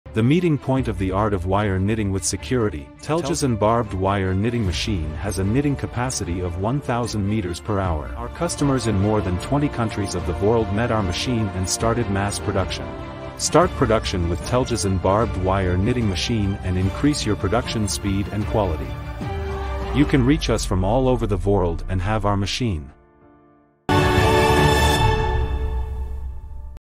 Barbed Wire Knitting Machine | Telcisan
Hello, in this video, we showed you the Telcisan brand Barbed Wire Knitting Machine. I hope you liked our video.

Welcome to Youtube Channel of Turkey's Biggest Machine Advertisement Site In this channel, we show you how the machines of the companies we work with work.